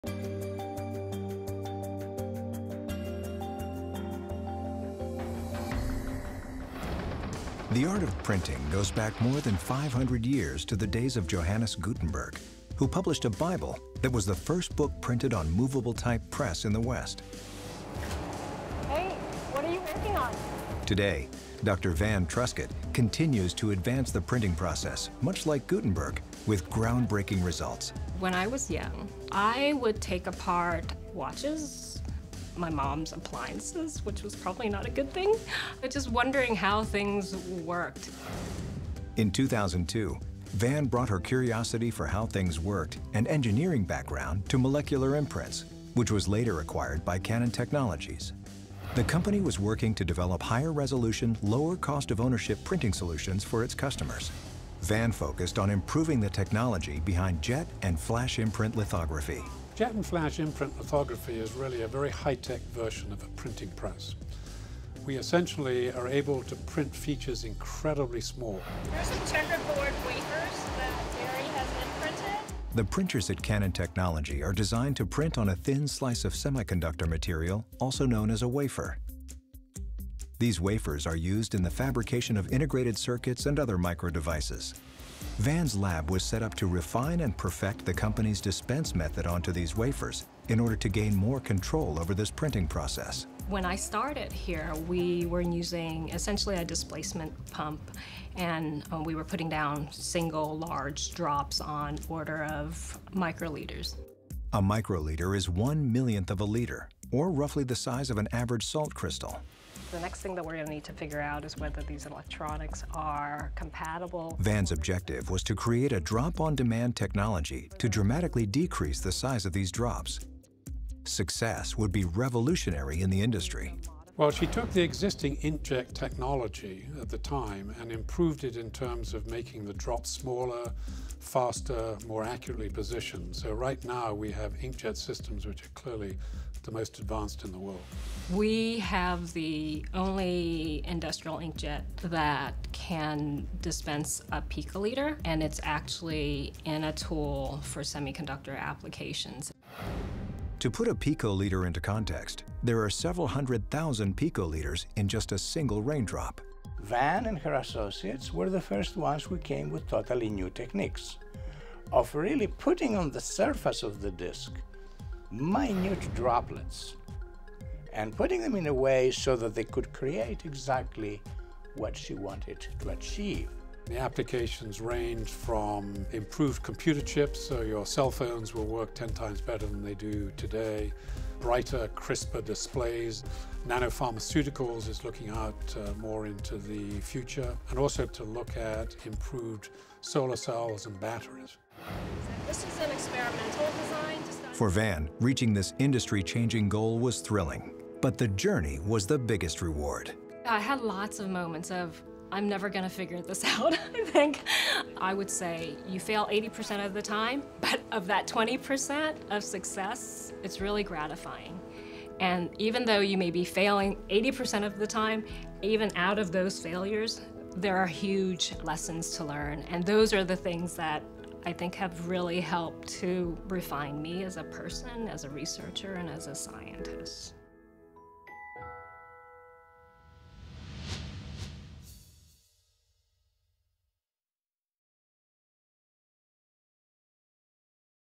TAMEST 2016 Edith and Peter O'Donnell Awards Tribute Video - Technology Innovation
Van N. Truskett, Ph.D., was the recipient of the 2016 Edith and Peter O'Donnell Award in Technology Innovation.